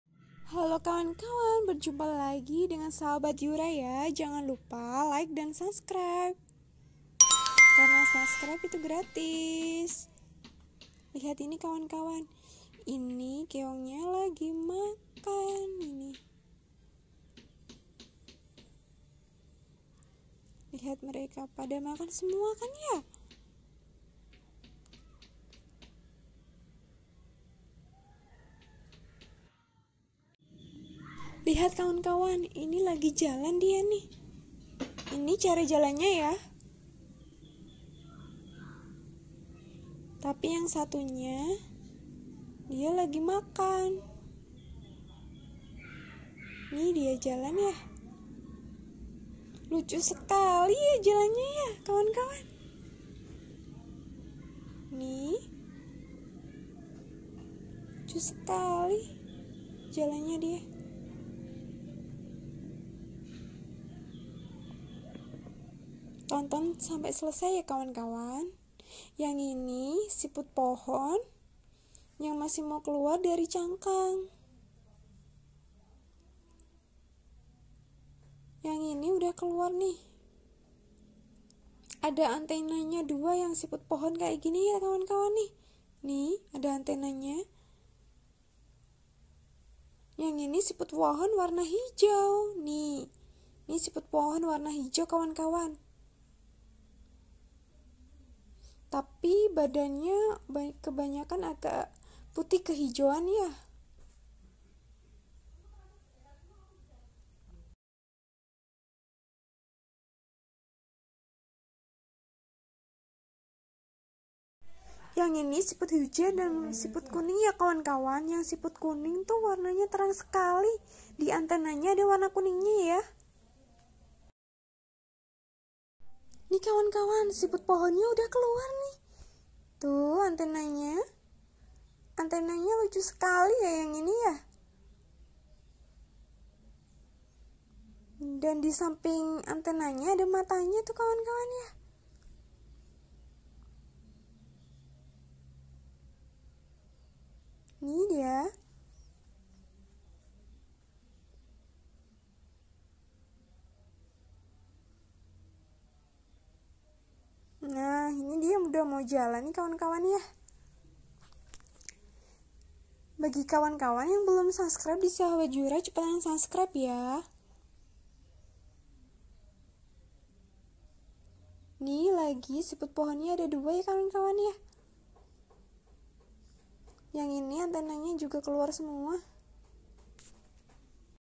Wow‼️ SIPUT KEONG MAKAN DAN BERJALAN ❗MELIHAT SECARA DEKAT WARNA @sahabatyura
melihat secara dekat warna siput dan keong cara makan, berjalanya dan warna siput

kawan-kawan.